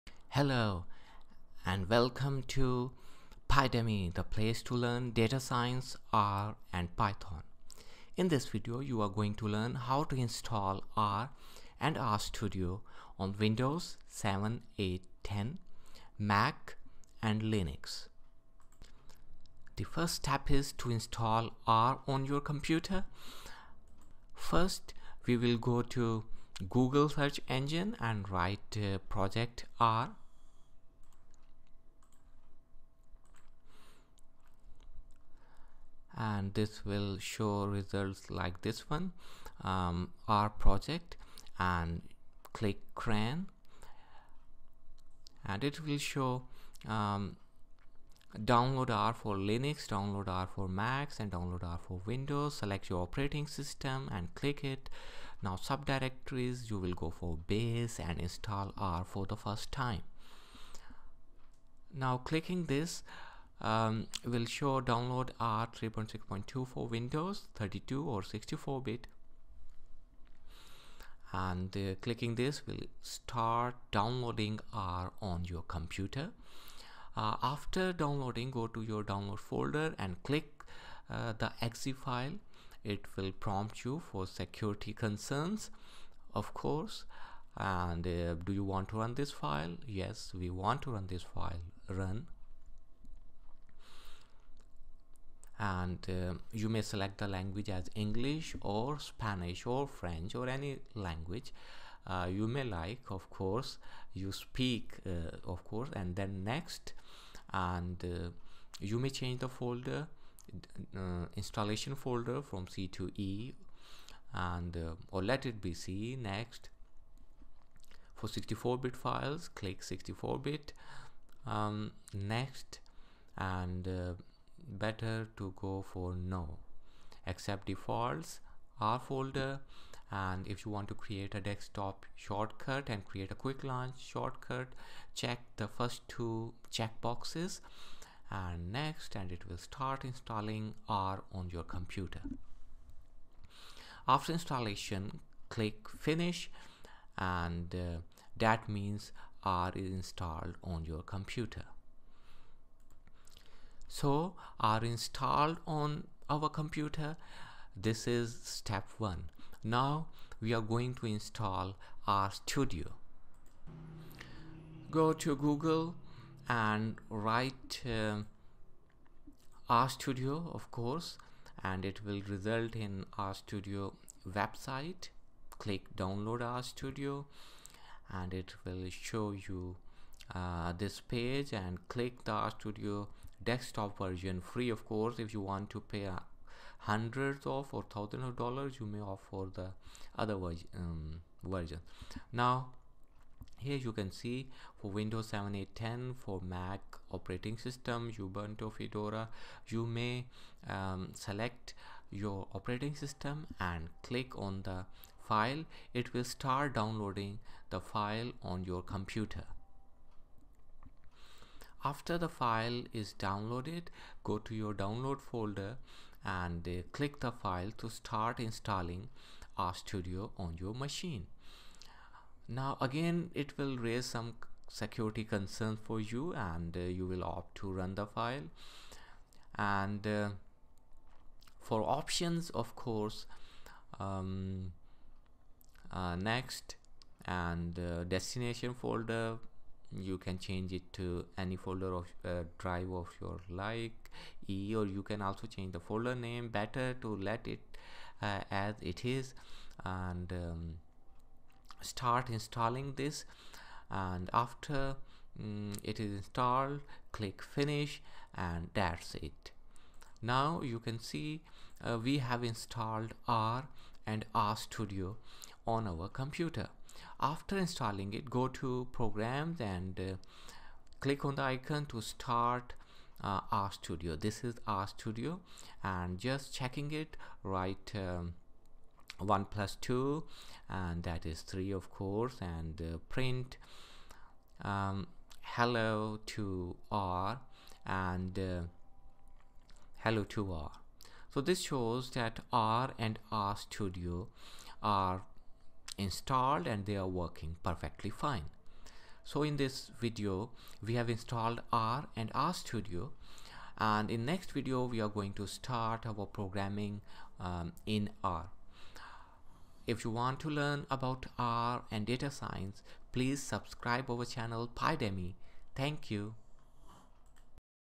R - Install R and R Studio on Windows, Mac, Linux (2020) - R Programming Tutorial for Beginners
This video will show you how to install R and RStudio on Windows 7,8,10 Mac and Linux Step by Step and Simple way. This video is part of Series - Learn R from scratch for Beginners - R tutorial for Beginners. Thanks.
Happy Learning R and Data Science.